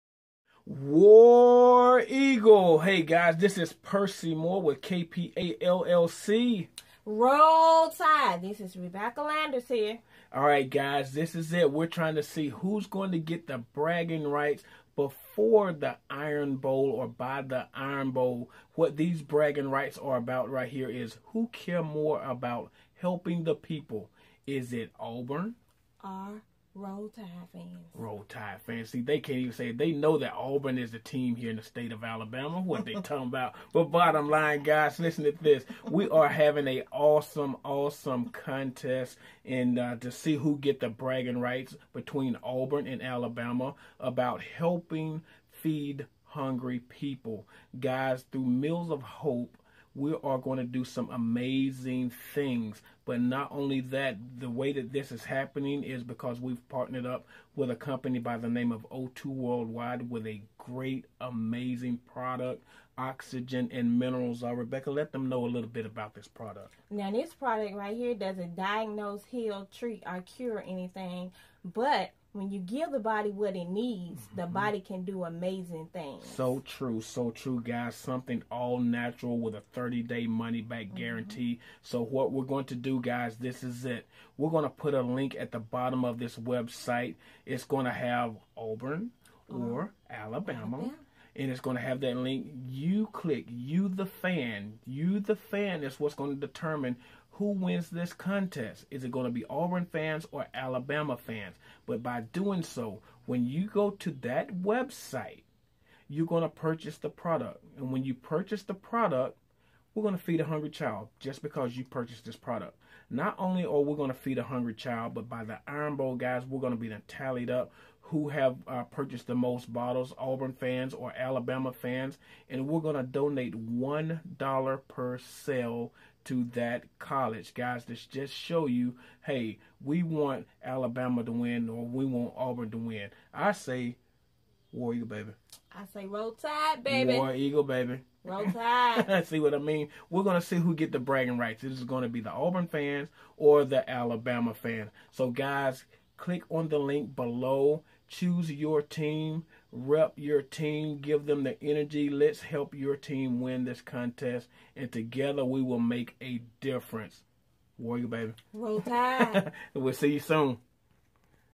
Alabama Fans V/S Auburn Fans
Bragging rights for who likes to help people more between Alabama Fans and Auburn Fans

Who's in it to win it!!!